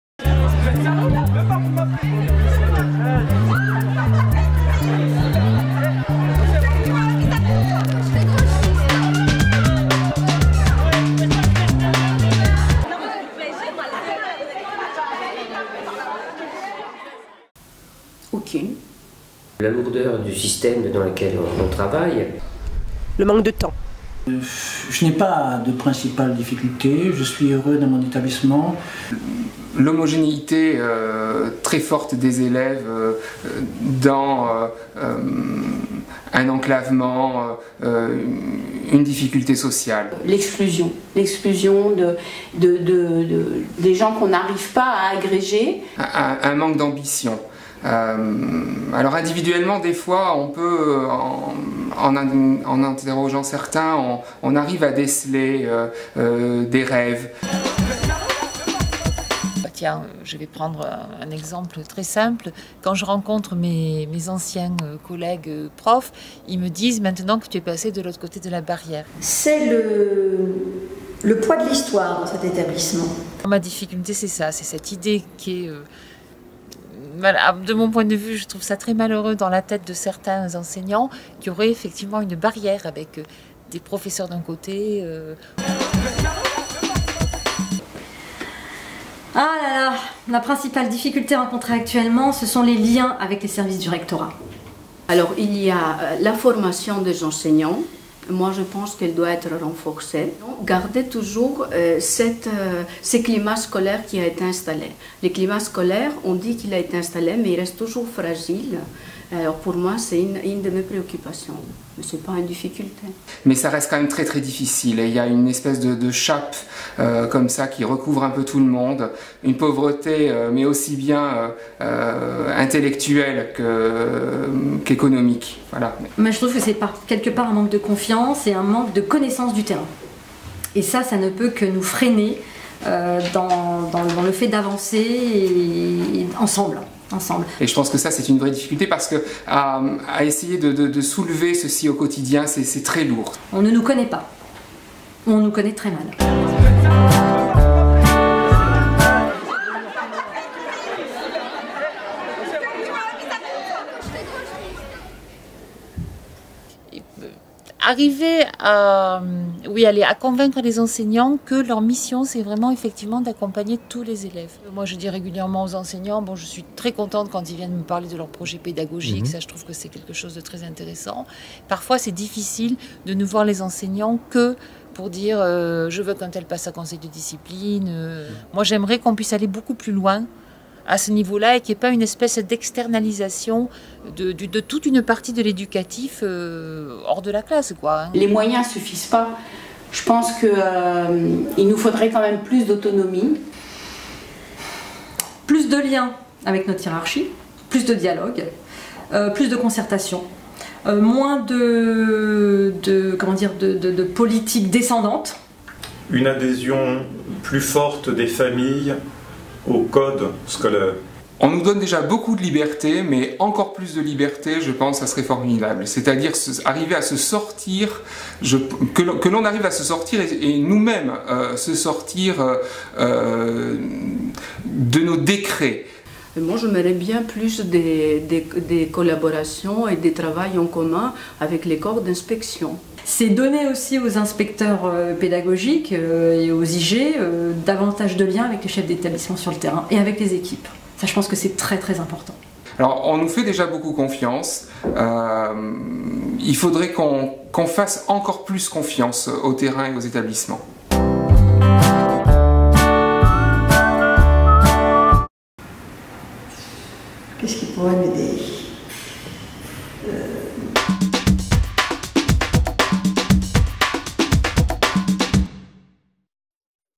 Paroles de chef (4) autour des pratiques de direction et de leadership scolaire
Dix questions Dix chefs d'établissement
épisode 4 : la principale difficulté rencontrée actuellement et qu'est-ce qui pourrait vous aider ?